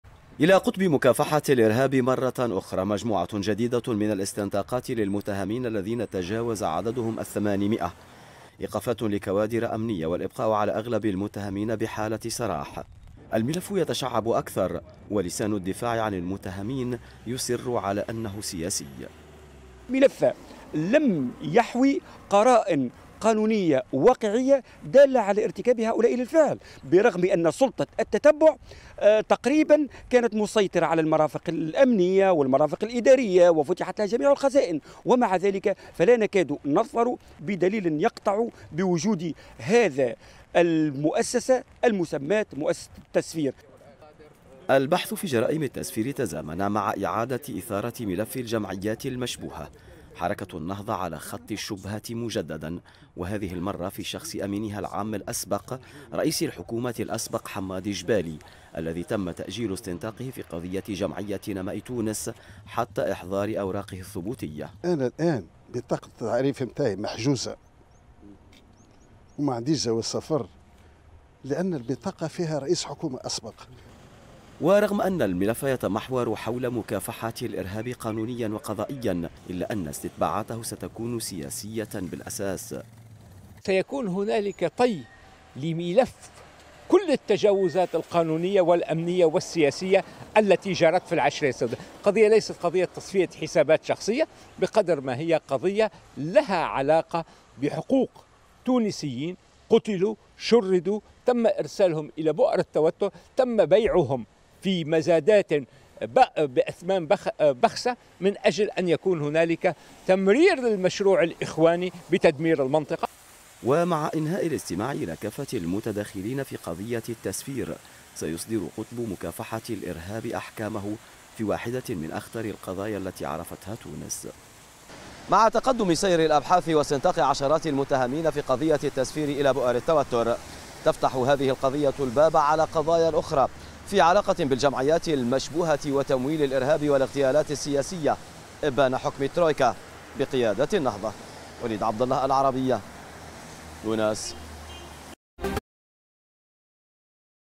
حبس كوادر أمنية وسياسية تونسية في قضية التسفير
مع استمرار التحقيقات في قضية التسفير.. قاضي التحقيق في تونس يصدر قرارًا بإيداع كوادر أمنية وسياسية بالسجن